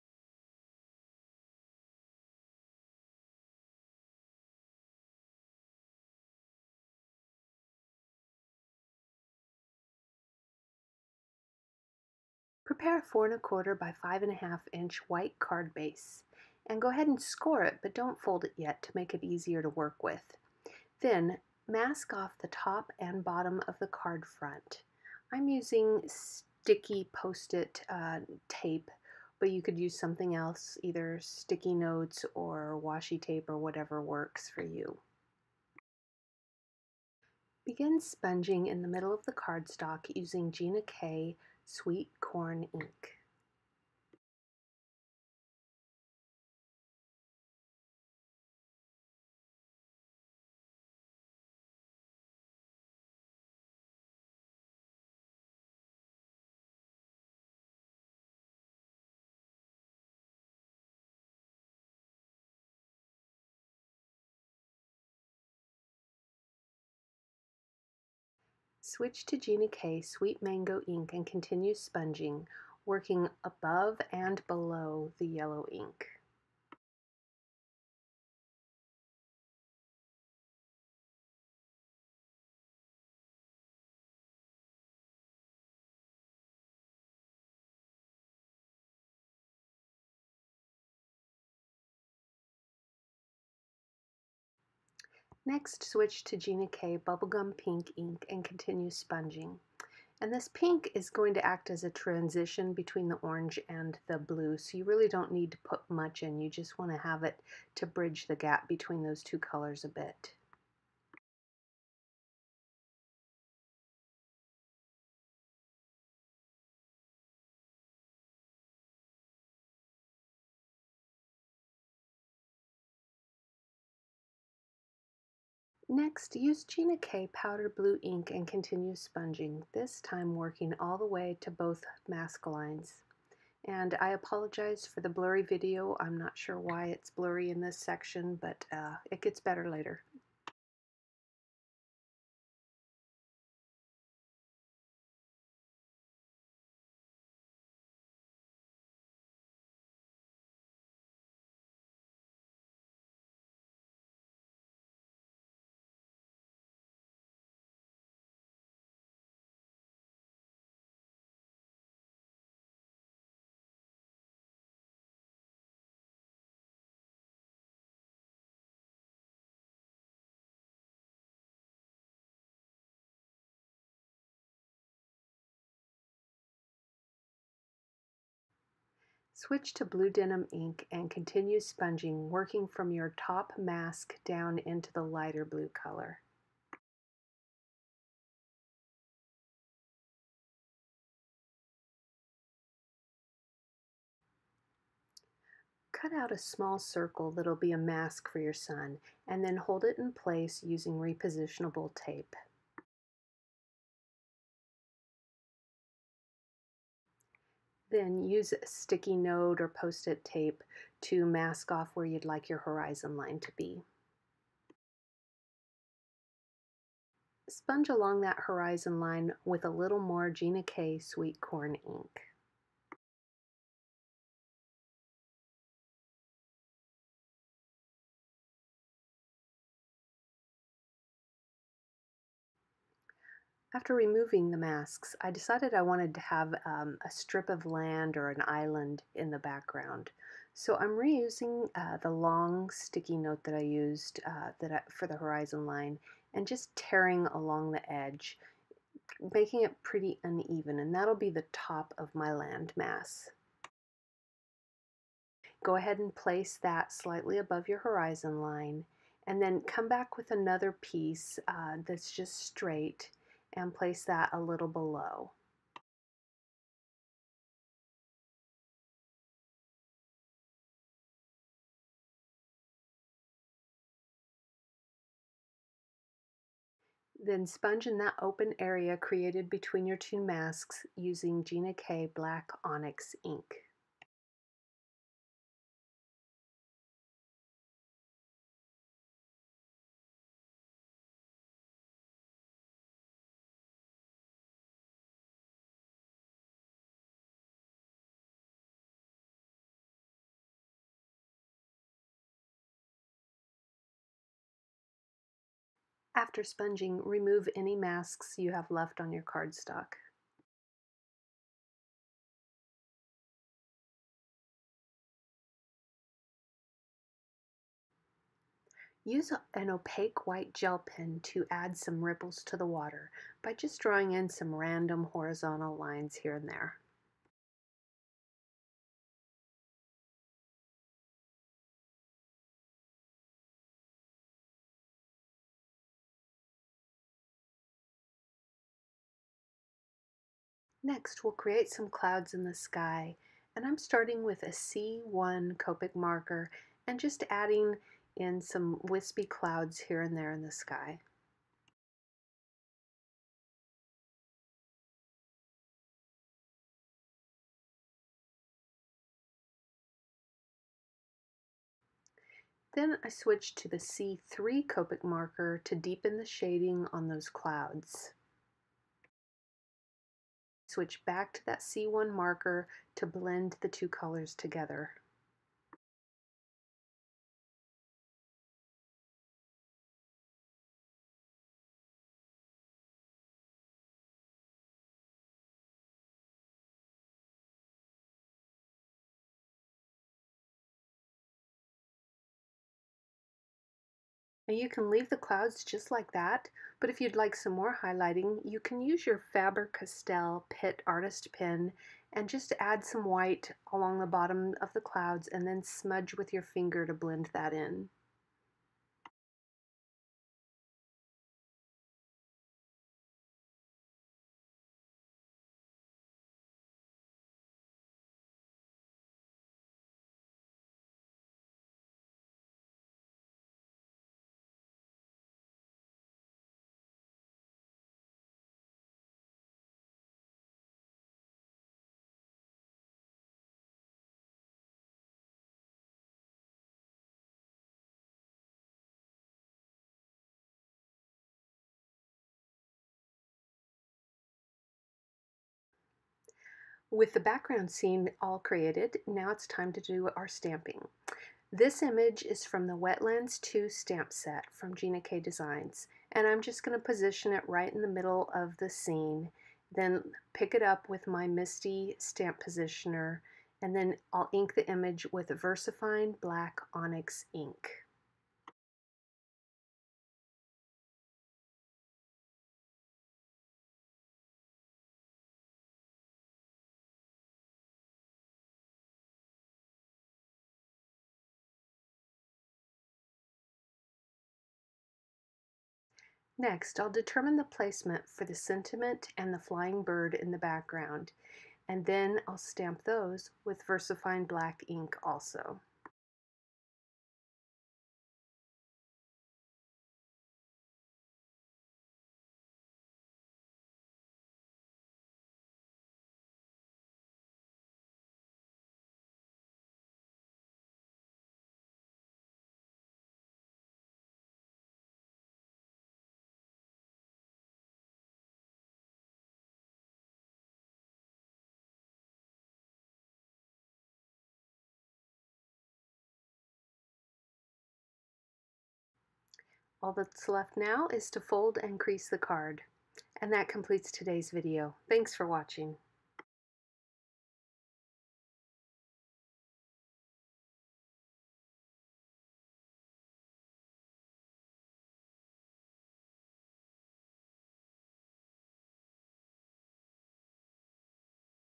Bird in Flight Card
Create a masculine card by creating a beautiful water scene. Featuring images from The Wetlands 2 stamp set from Gina K Designs.

Finished Card Size - 4 ¼ by 5 ½ inches

Supplies

 The Wetlands 2 Stamp Set - Gina K Designs

White Cardstock - Gina K Designs

Powder Blue, Blue Denim, Sweet Corn, Sweet Mango, Bubblegum Pink Inks, Black Onyx- Gina K Designs

Versafine Onyx Black Ink - Gina K Designs

Jumbo Sponge Daubers - Gina K Designs